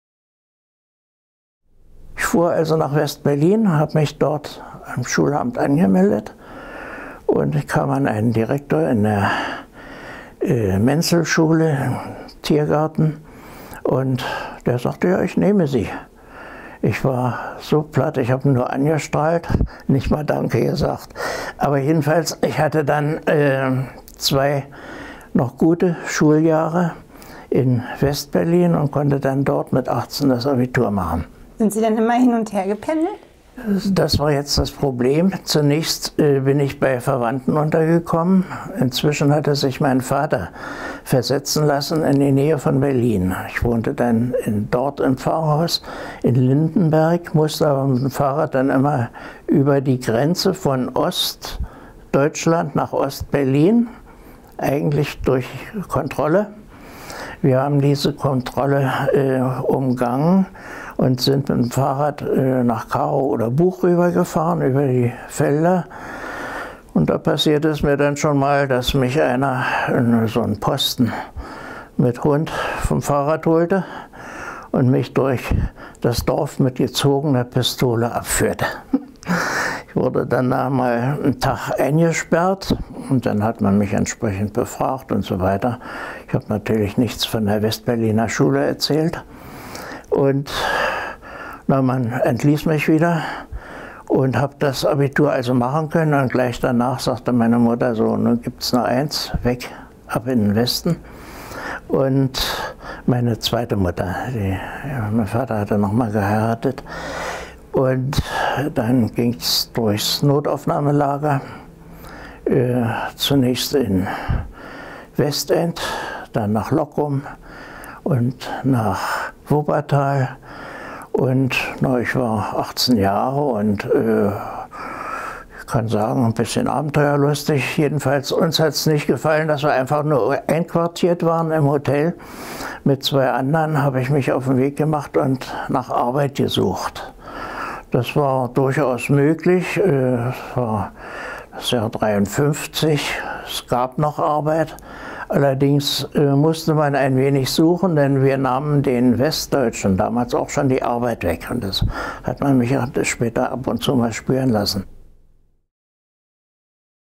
Hartmut Kieburg: "Ab in den Westen!"
Der DDR-Bürger Hartmut Kieburg wurde 1948 wegen "staatsfeindlichem Verhalten" von der Schule verwiesen. Daraufhin machte er in West-Berlin sein Abitur. Gemeinsam mit seiner Stiefmutter und seinen Geschwistern floh Kieburg Anfang der 50er Jahre in die Bundesrepublik.